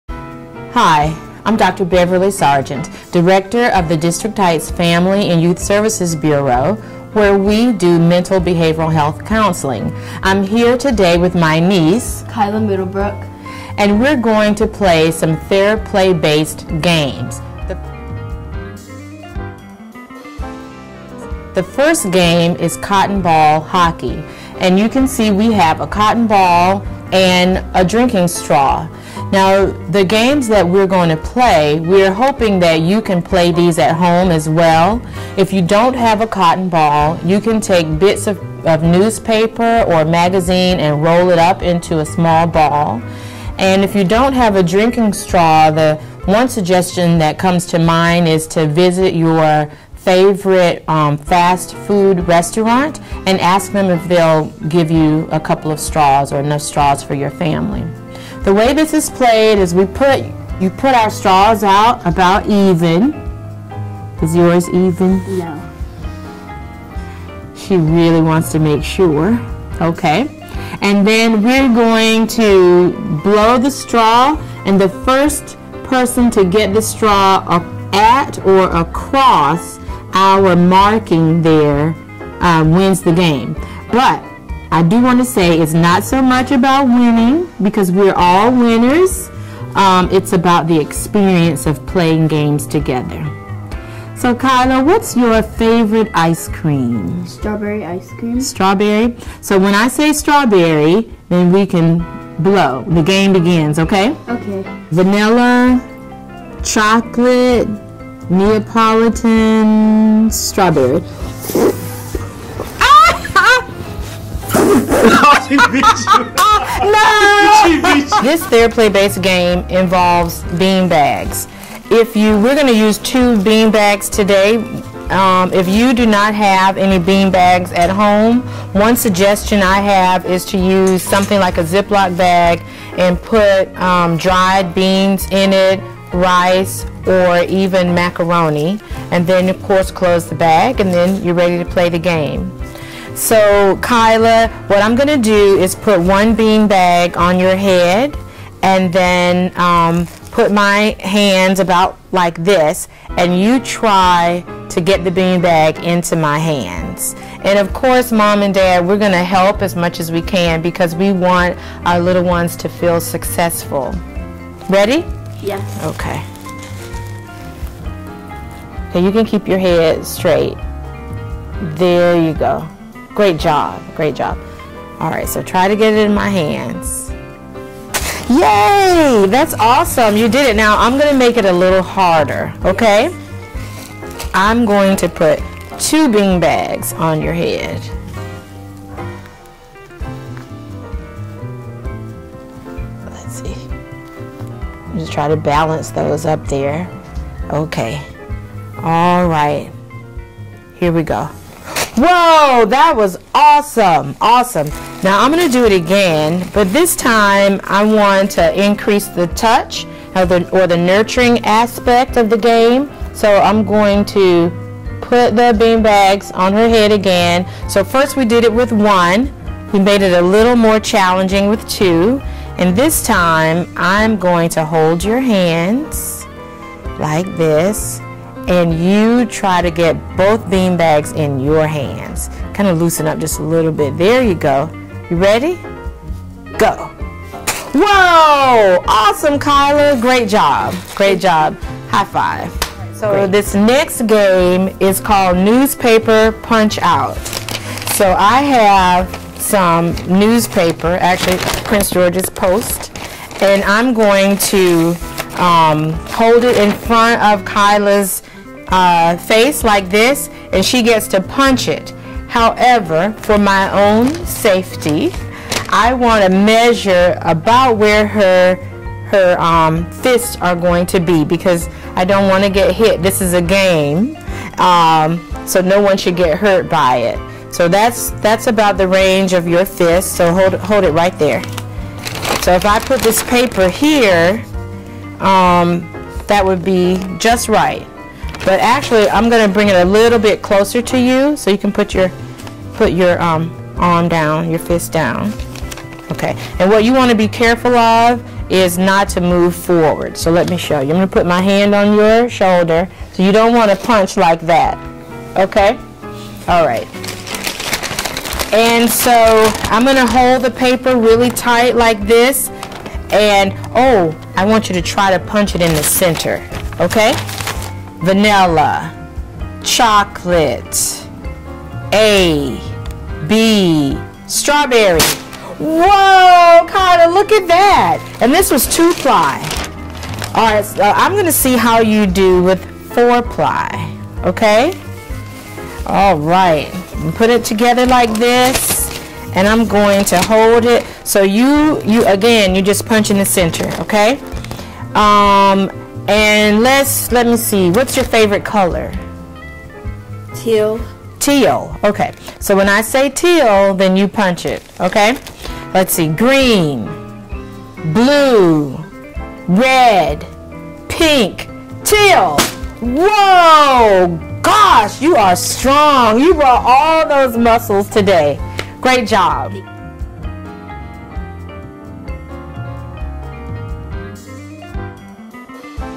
Theraplay-based Activities for Your Family #5
Here are a few Theraplay-based activities you can do with your family!
1. Cotton ball hockey
2. Bean bag game
3. Newspaper punch